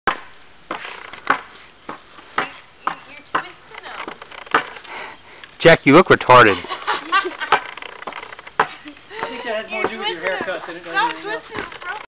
Jack walks on the stilts and is lauded
Camera man Josh offers constructive criticism that is heartfelt and sincere.  I was about to cry.